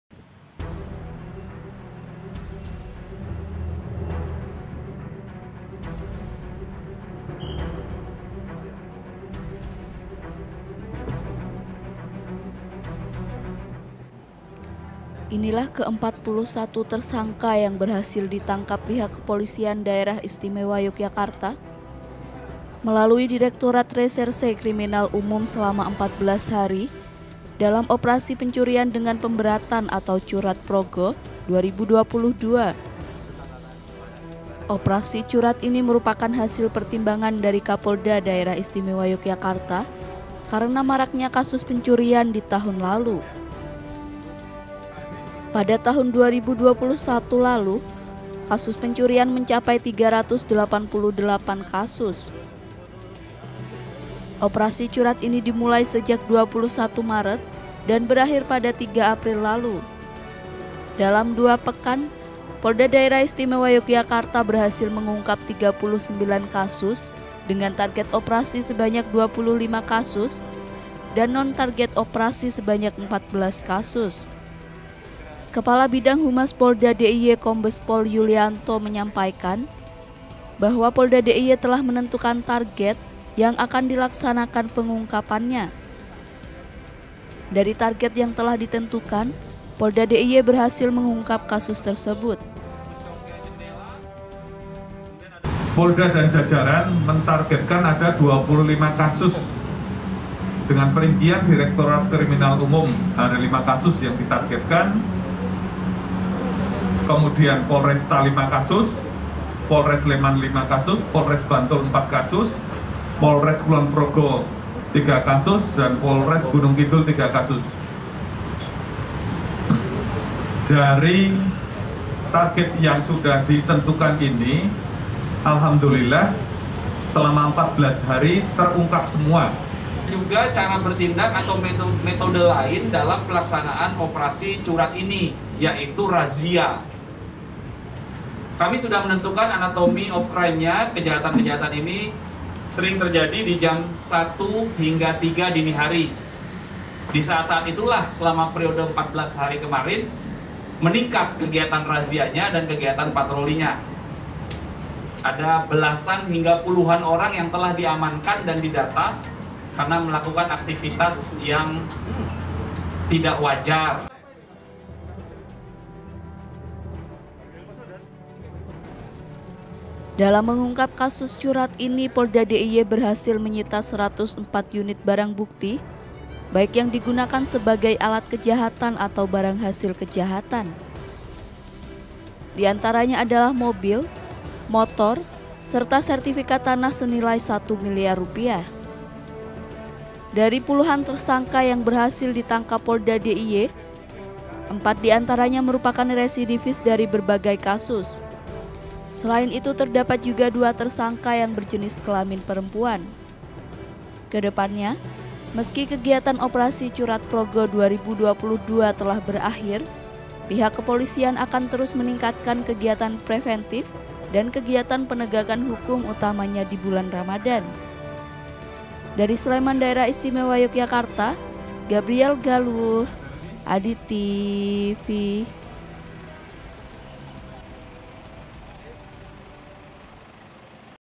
GELAR OPERASI CURAT DUA PEKAN, POLDA DIY TANGKAP 41 TERSANGKA
Selama empat belas hari, operasi curat progo dua ribu dua puluh dua berlangsung, pihak kepolisian daerah istimewa yogyakarta, berhasil mengungkap tiga puluh sembilan kasus. Dari kasus tersebut, kepolisian juga berhasil meringkus empat puluh satu tersanga, serta ratusan barang bukti, yang ditaksir senilai 1 milyar rupiah.